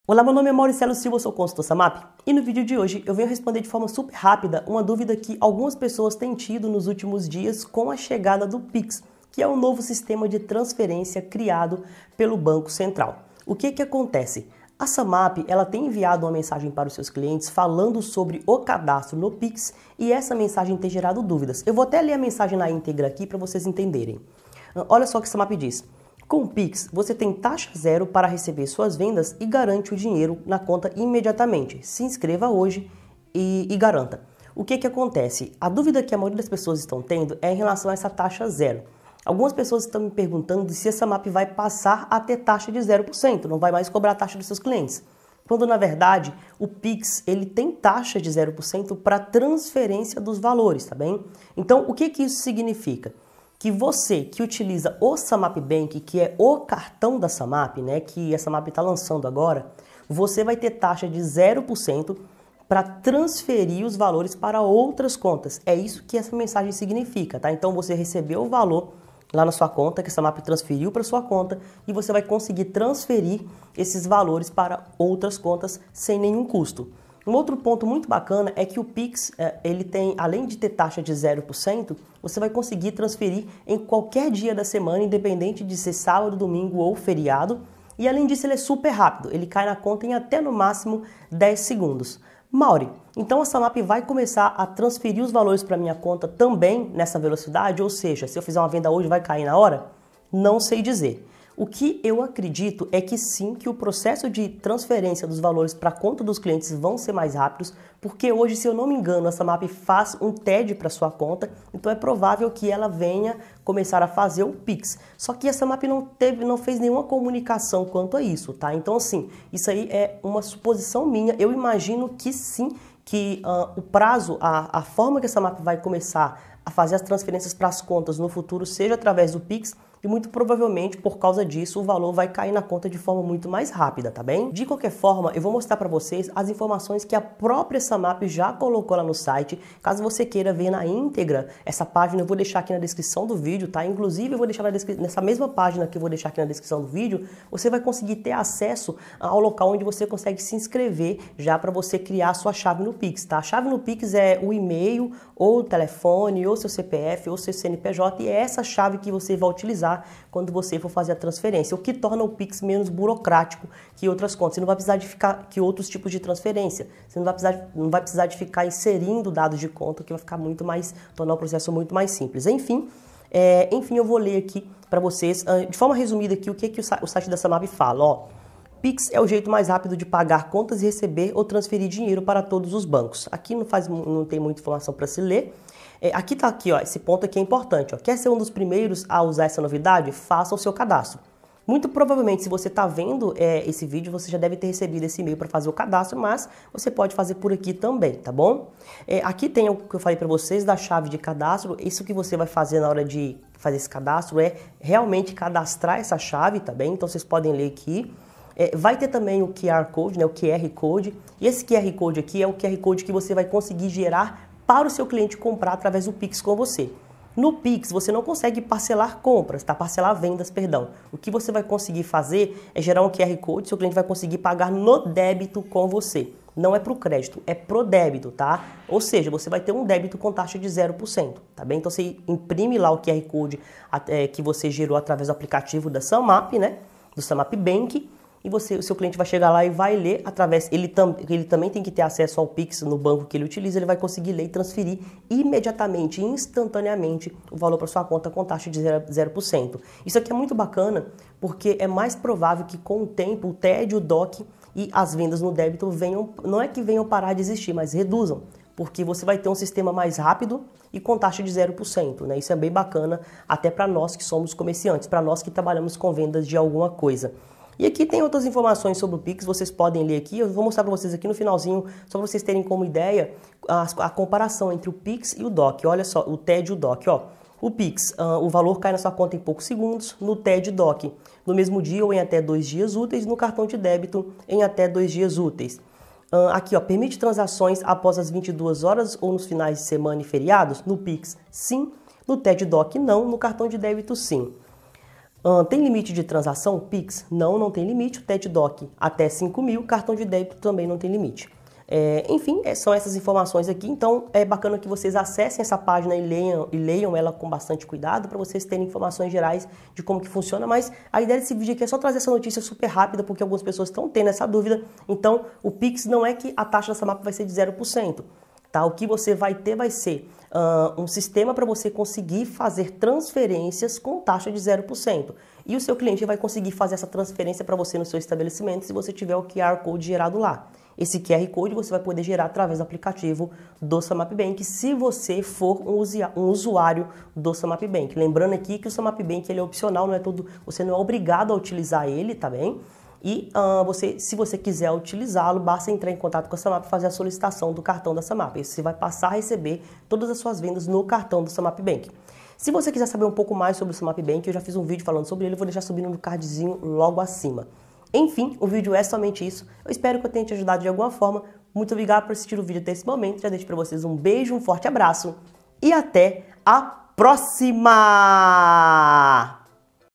O PIX vai ACABAR com as TAXAS da SumUp?
O PIX está prestes a ser lançado e com isso, dúvidas sobre seu funcionamento tem sido comum; Nesse vídeo iremos falar sobre o impacto do PIX nas taxas e no tempo de recebimento das vendas na SumUp; O que vai acontecer?